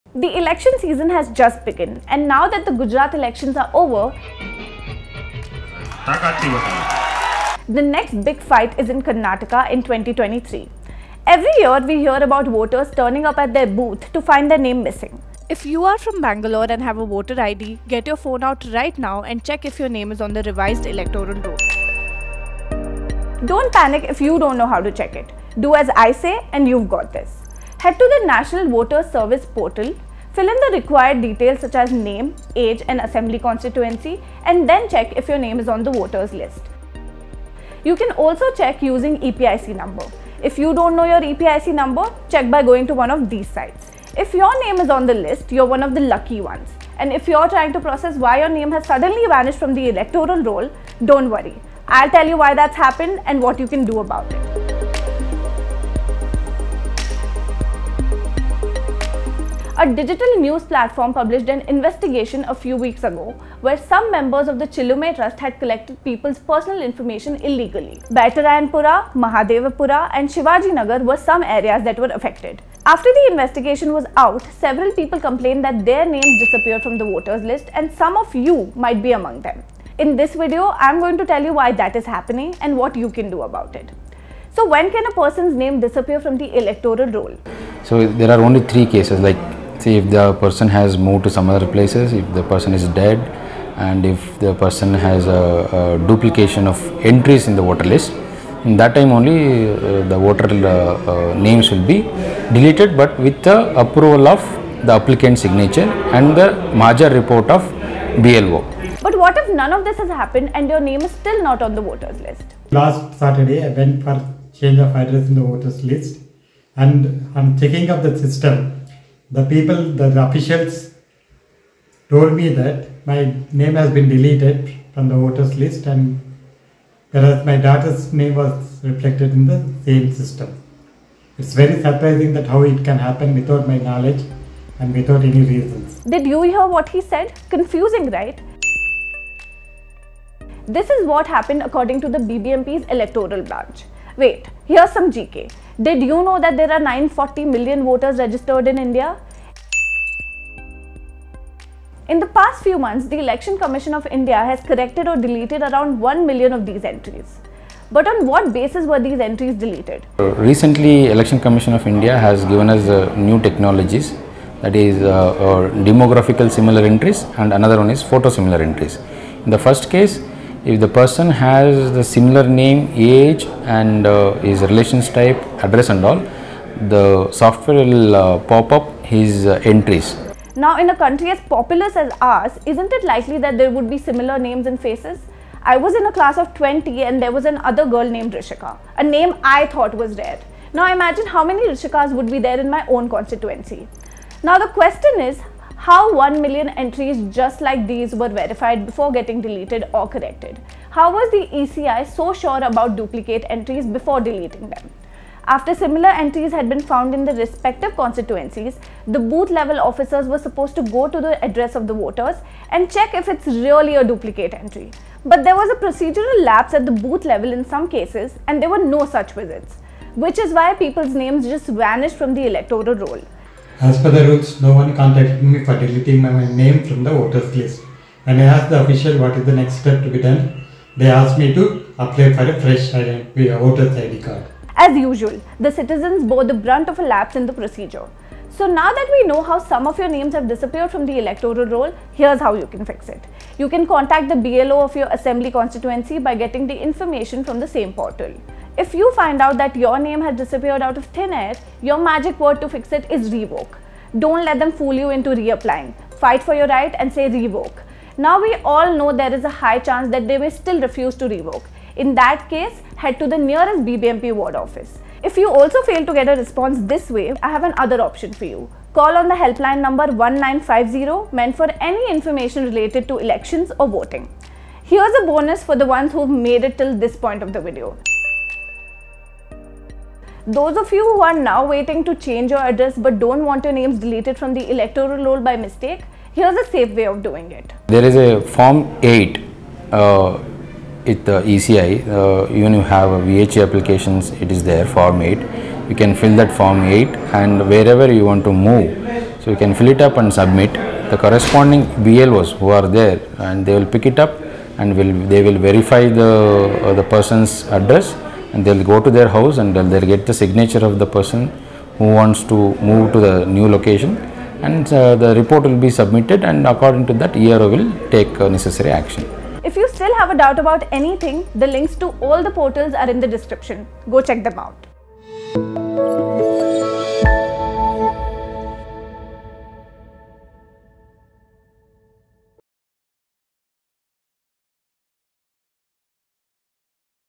Name missing from voters list in Bengaluru? This is why
Have you checked if your name is on the revised electoral roll ahead of Karnataka elections in 2023? Names of some voters in Bengaluru have gone missing from the voters list. Rishika Kashyap tells you why that is happening, and what you can do about it.